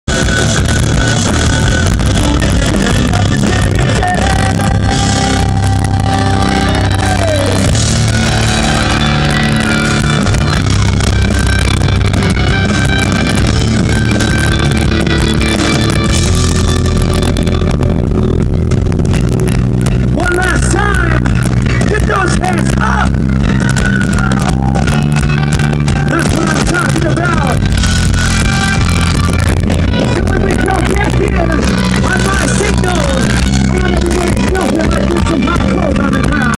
There for tomorrow @ warped tour 2009
There for tomorrow warped tour 2009 in salt lake city, utah